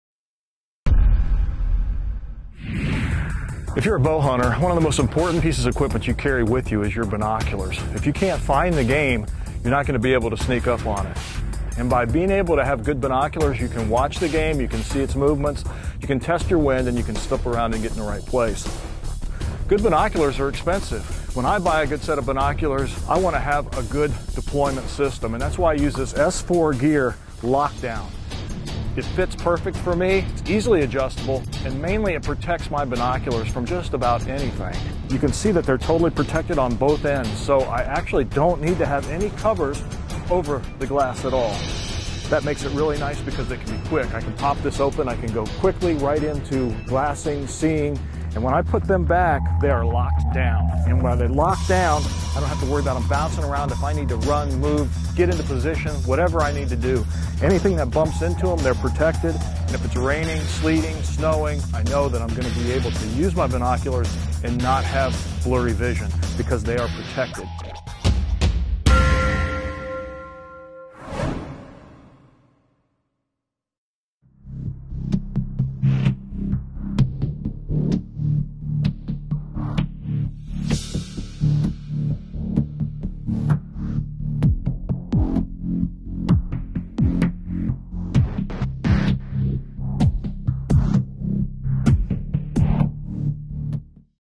How To Protect Your Binoculars With The S4 Gear Lock Down
Adventure Bowhunter Tom Miranda explains why he uses the S4 Gear Lock Down to protect his binoculars while on a hunt.